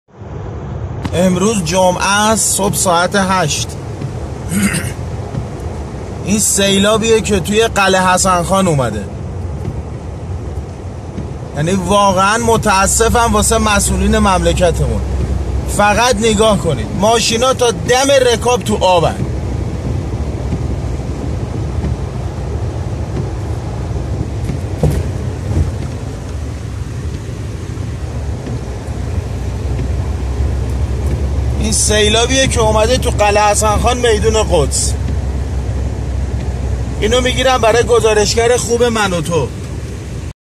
آبگرفتگی شدید معابر در شهر قدس
آبگرفتگی شدید معابر در قلعه حسن‌خان، شهر قدس